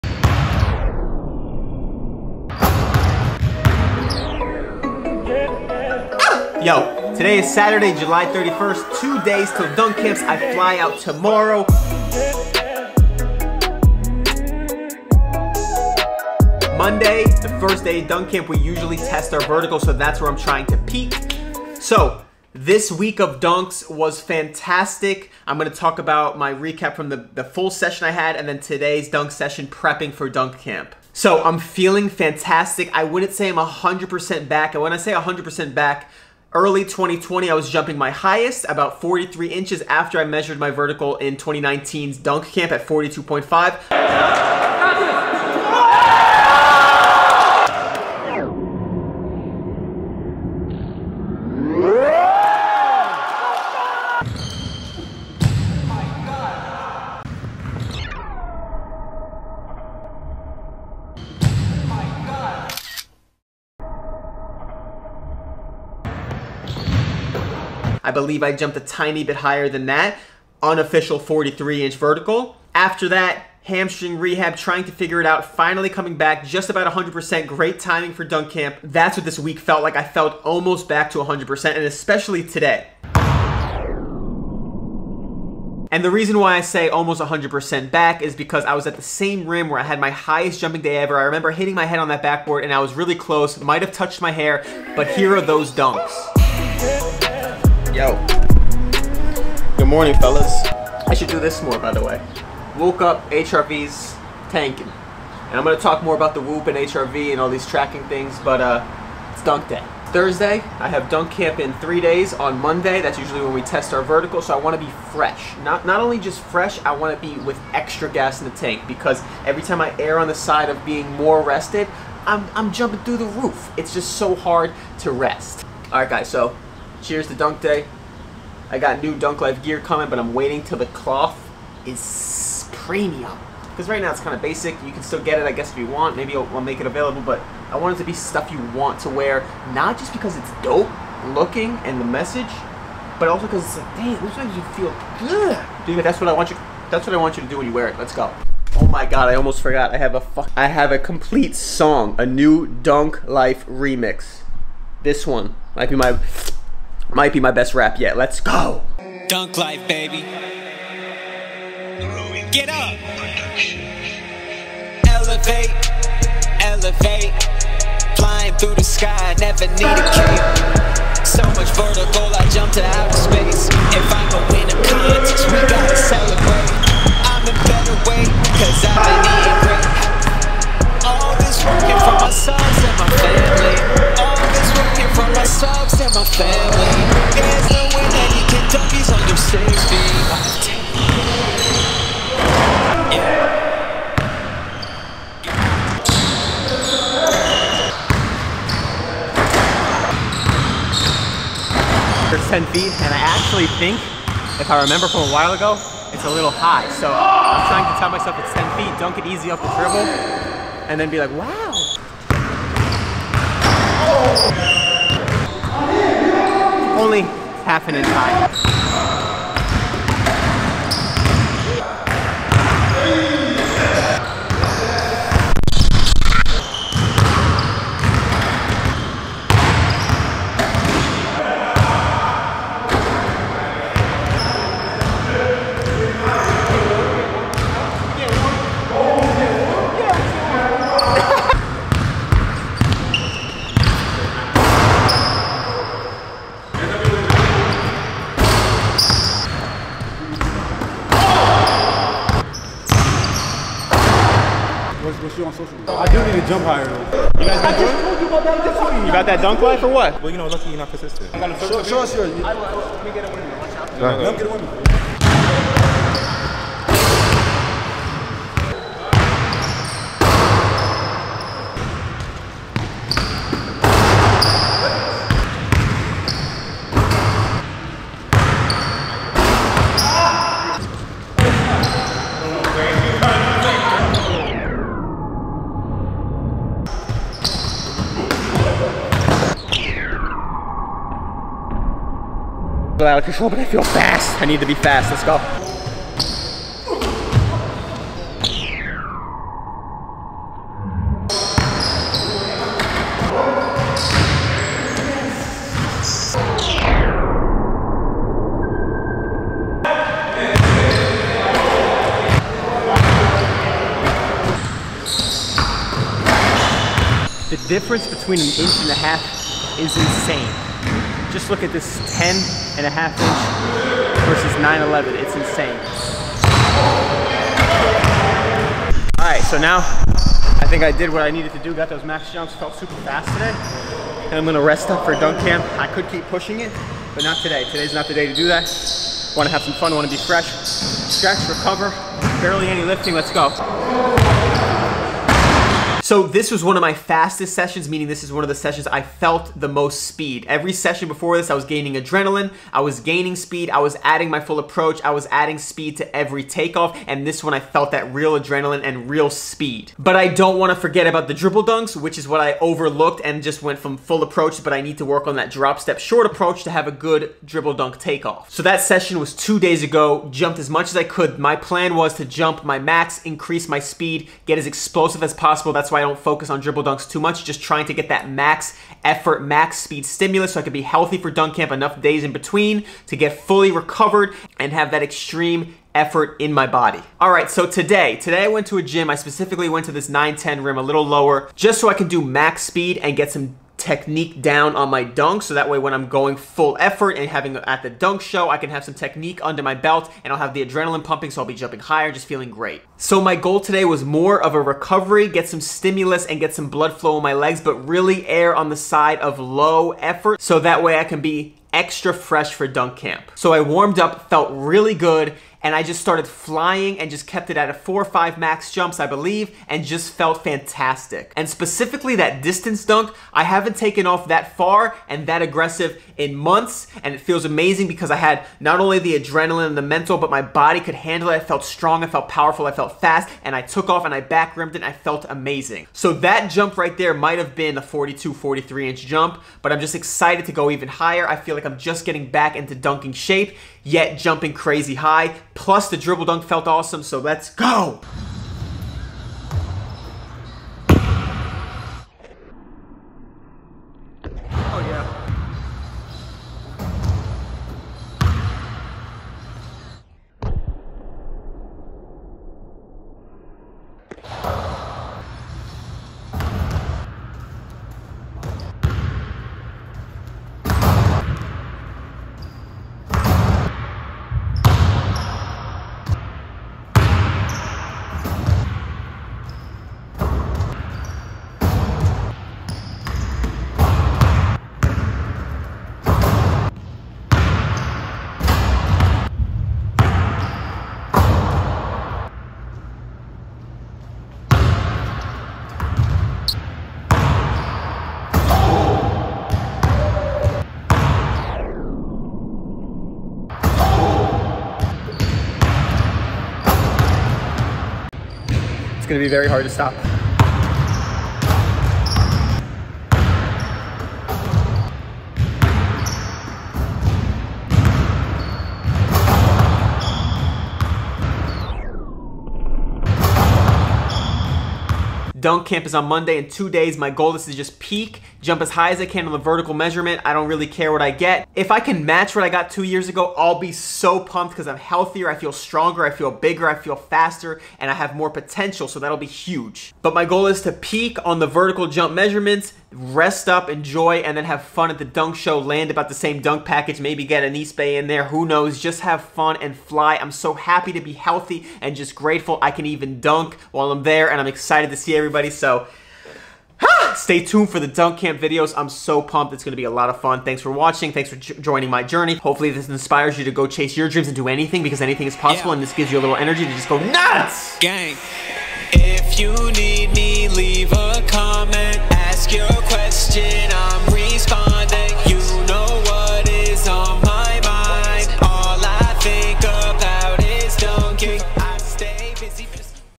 Back to 42" VERT?! 5'10" Dunk Camp Prep! [ Dunk Journey Vlog 513]
FLYING!! FEELING AMAZING ask any questions at all
just happens to be good timing
WE SHALL SEE!!

🍼Supplements StevenCeli.com/pages/gear

My stats:
🙋🏼‍♂️Standing Reach = 91” inches
👱🏼‍♂️my skin = 5’9.3” Barefoot (5'10")
🏀Vert ~ 42.5" max measured vertical